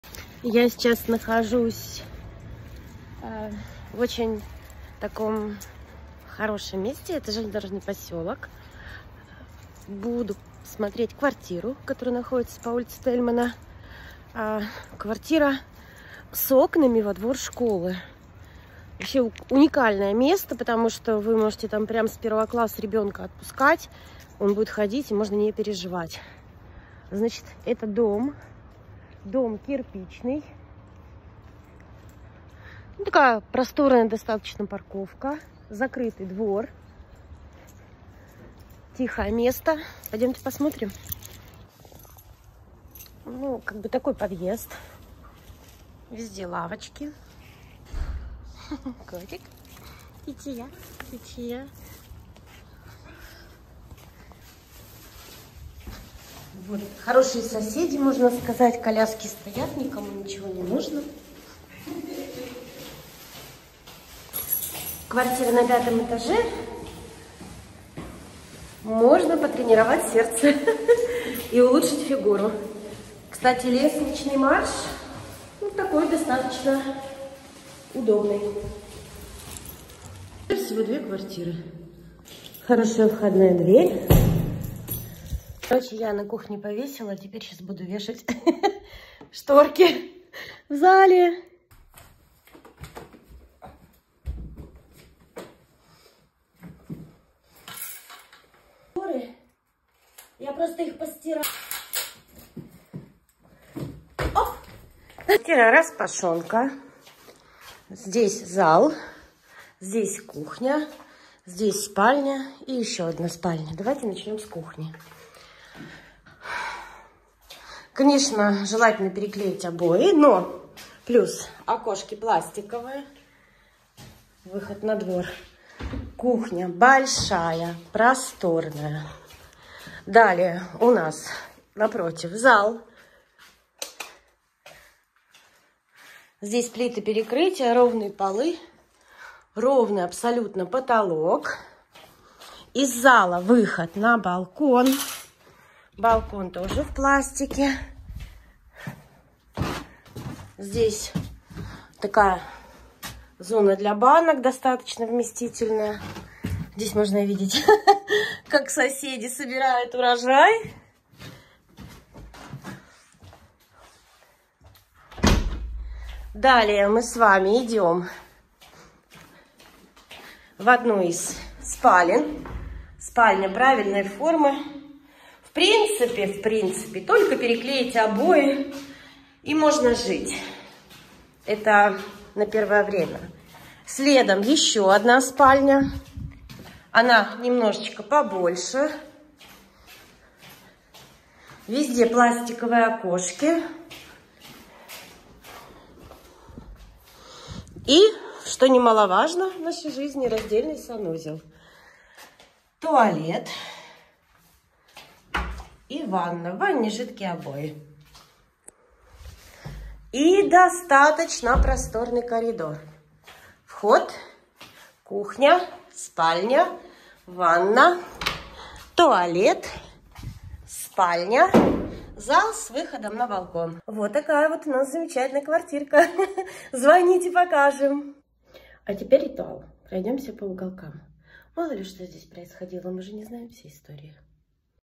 3 октября 2023 г.
В продаже просторная 3ка с изолированными комнатами по доступной цене
Тельмана 17 - Белгород ✔️